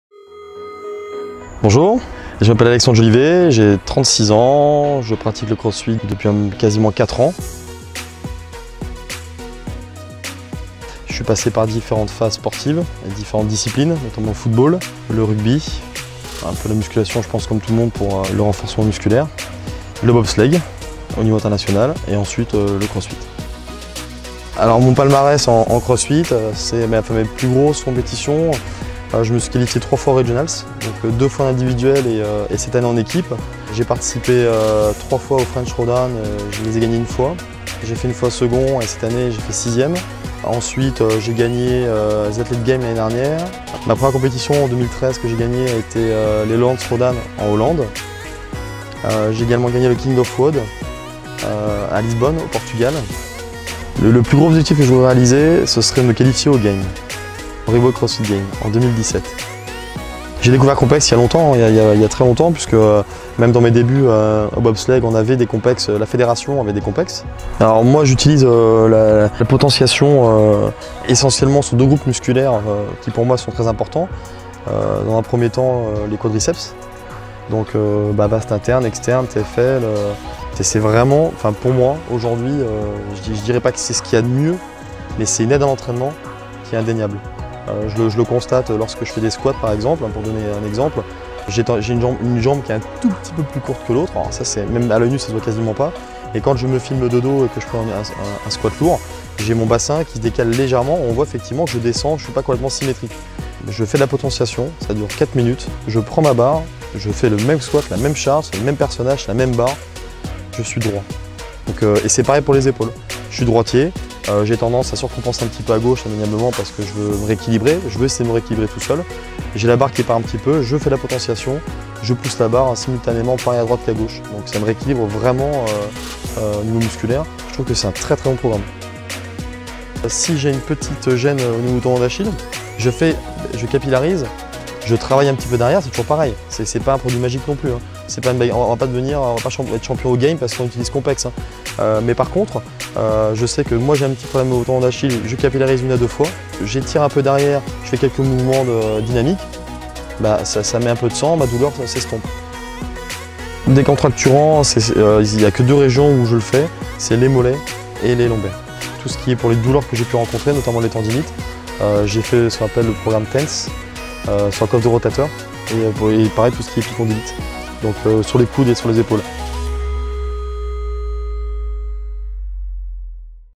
Alexandre Jolivet X Compex / Entraînement de Champion
Découvrez comment Alexandre Jolivet, crossfiteur, utilise son Compex.
Programme Potentiation avant un un Wod permet de booster la vitesse de contraction musculaire
Programme Capillarisation améliore la résistance musculaire, augmente le débit sanguin
Programme Décontracturant permet de diminuer de la tension musculaire, réduit les douleurs musculaires.
Programme Tens pour ses diminuer les douleurs localisées aiguës ou chroniques.